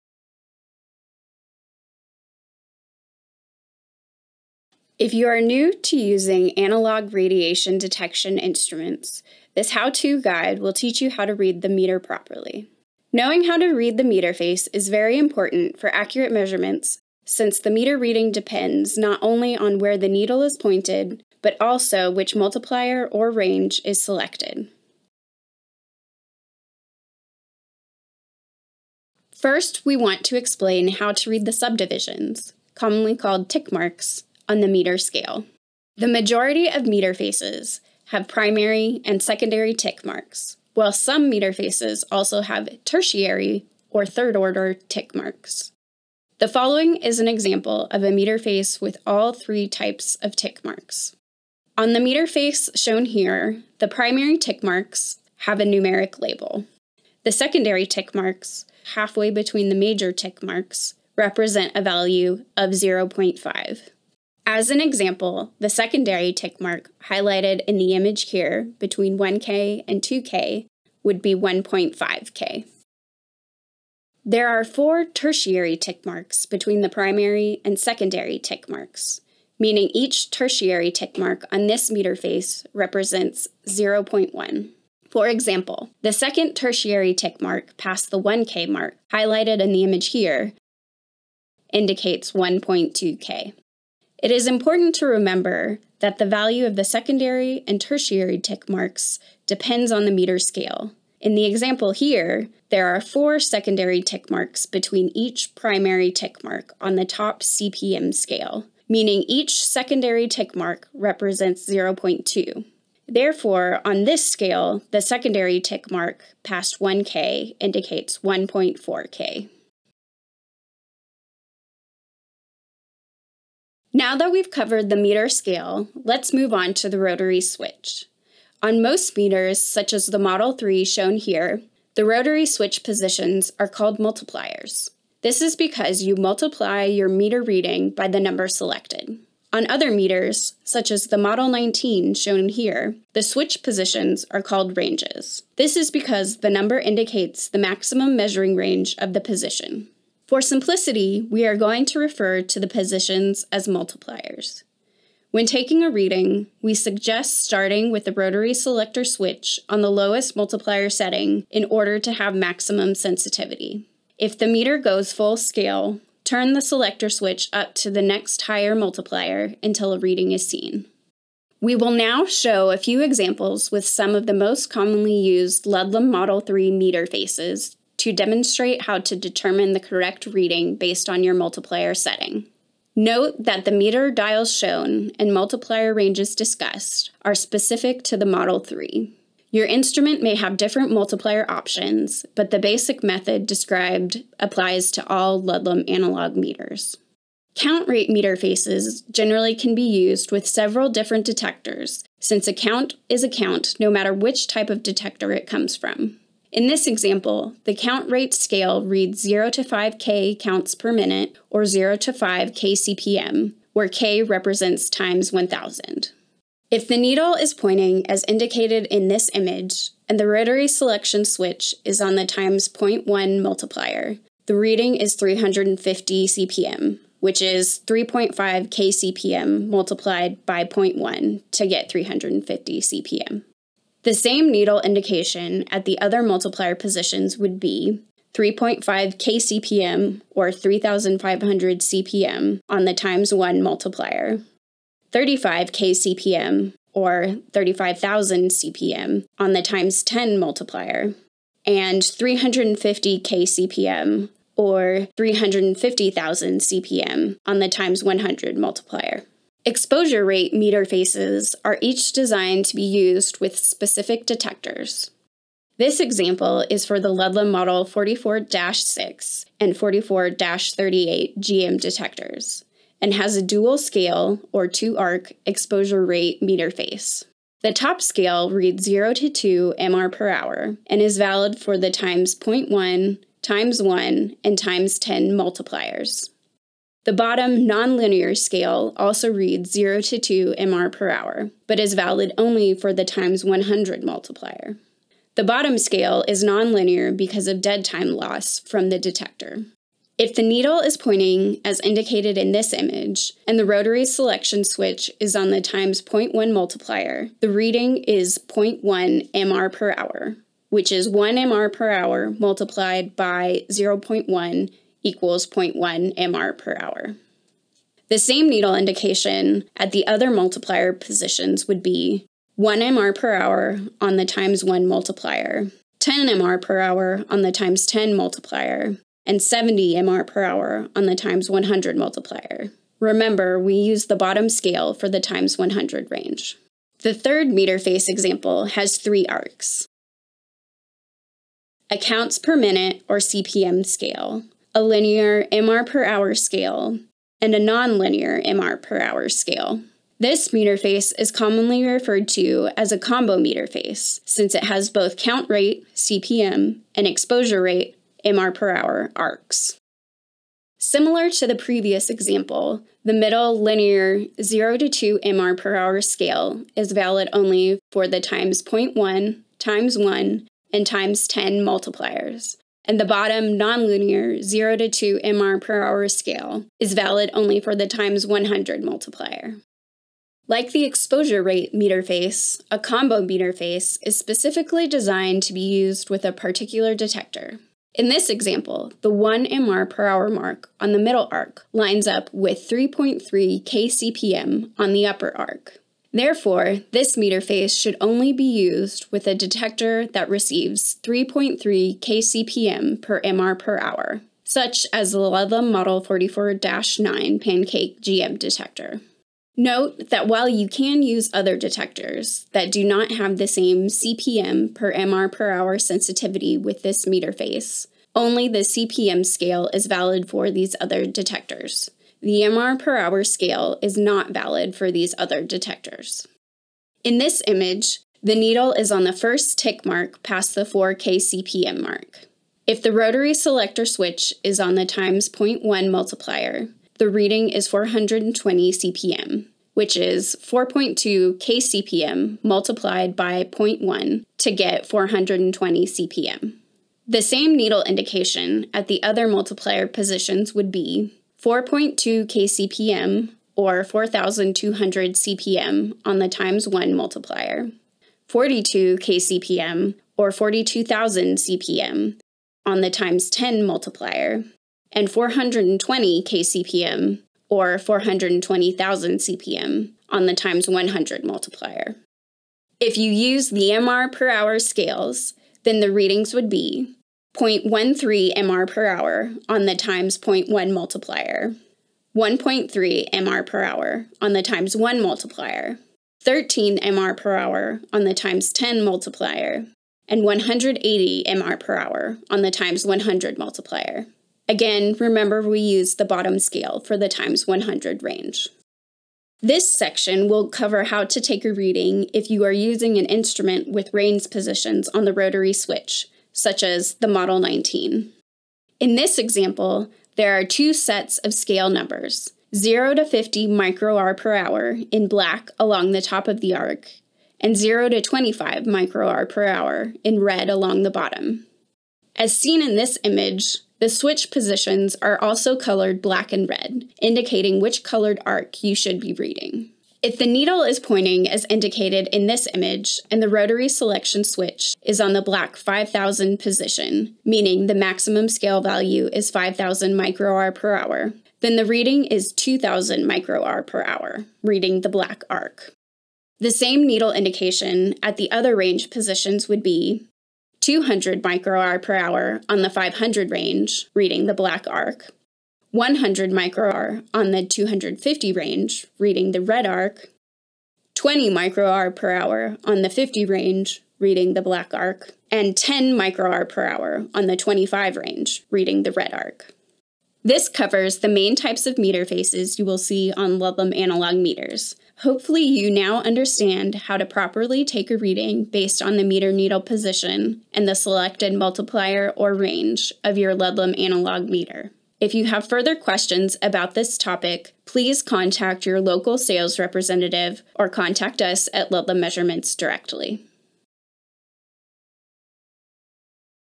How To Read an Analog Meter Dial
If you are new to using analog radiation detection instruments, this how-to guide will teach you how to read the meter properly.

This video covers the three main types of meter faces used with instruments that have a multiplier selector switch, demonstrated with the Ludlum Model 3. A final example shows how to read an instrument with a range selector switch, demonstrated with the Ludlum Model 19.

A written version of this video is available on our

Chapters:
0:00 Introduction
0:26 How to Read Tick Marks
2:01 Meter Multipliers and Ranges
3:17 Count Rate Meter Faces
4:19 Exposure Rate Meter Faces
5:37 Combo Meter Faces
8:19 Instruments with Range Settings